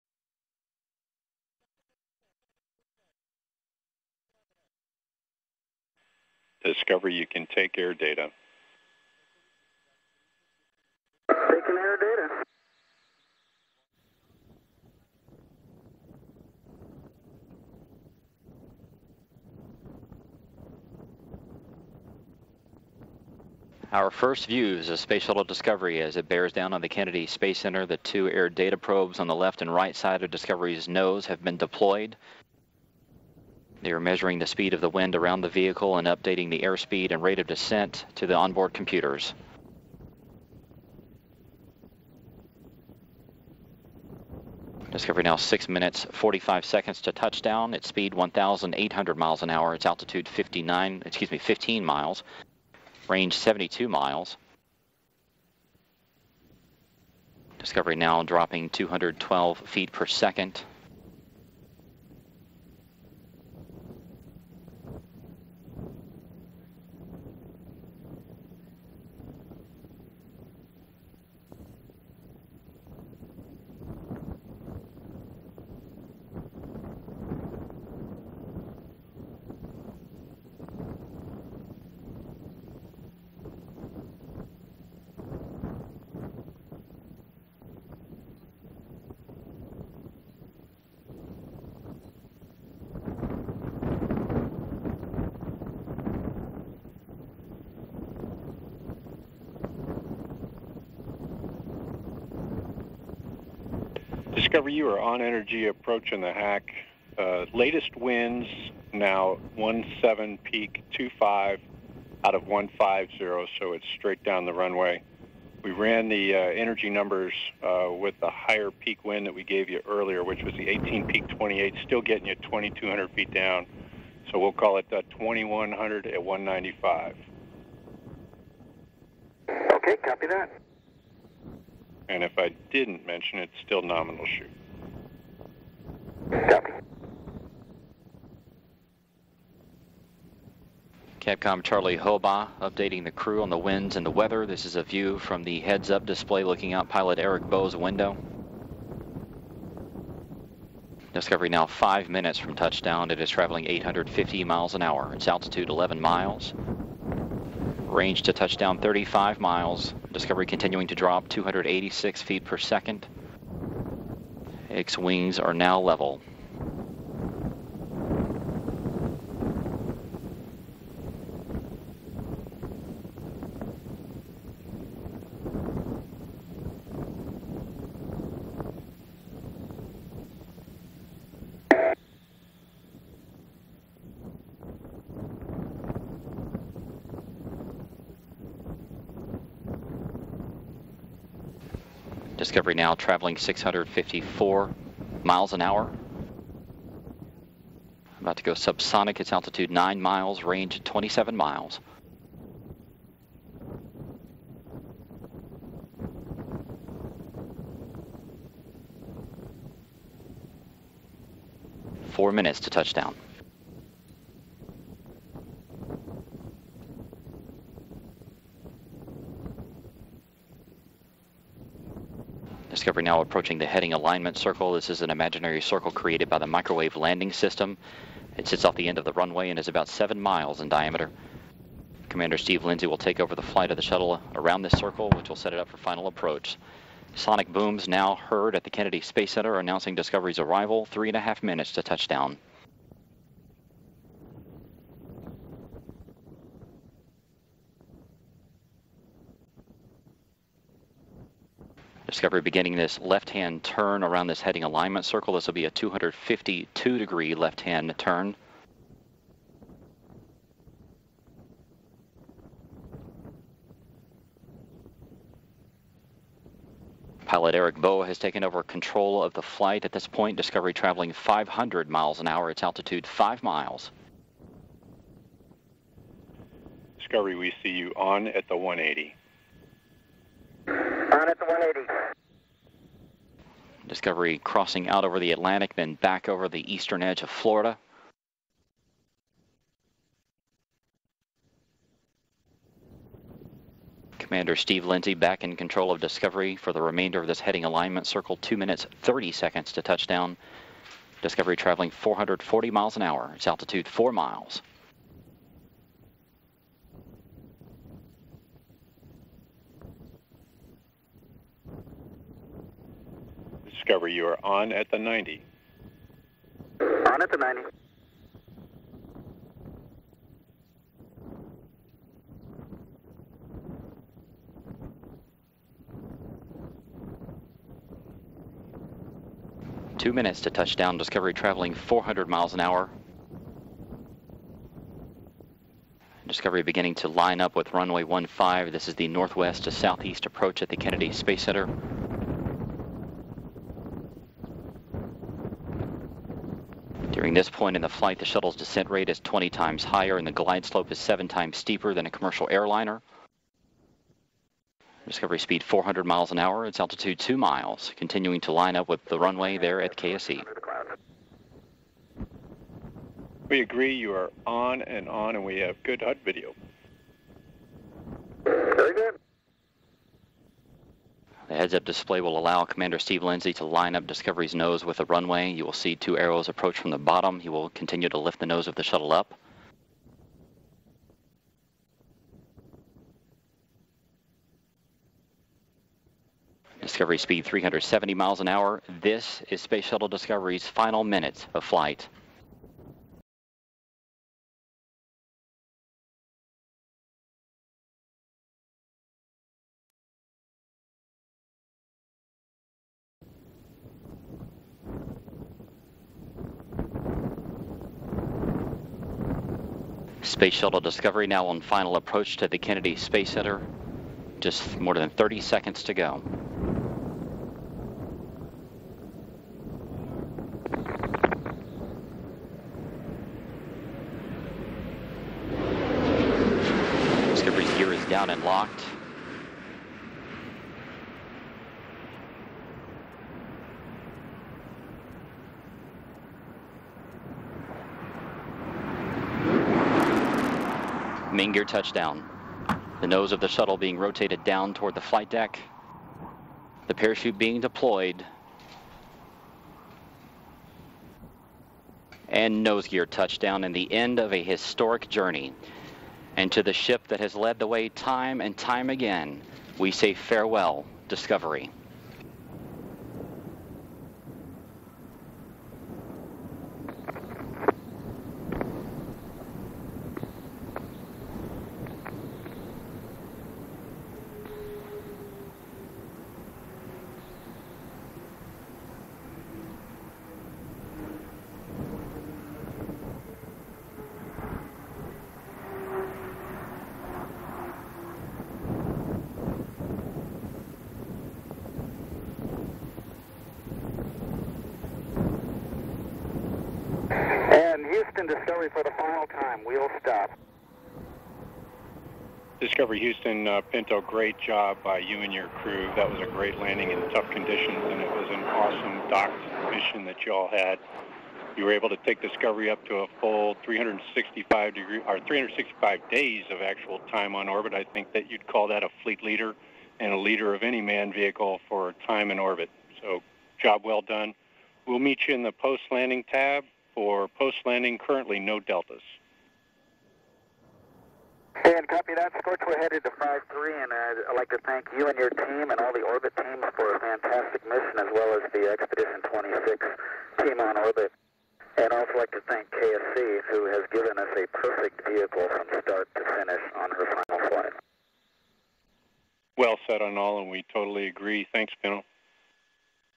STS-133 Lands at Kennedy Space Center for the last time
The final landing of Space Shuttle Discovery at the Kennedy Space Center. OV-103 has flown over 365 days in space in total. This final landing took place on March 9th, 2011 concluding the success mission of STS-133.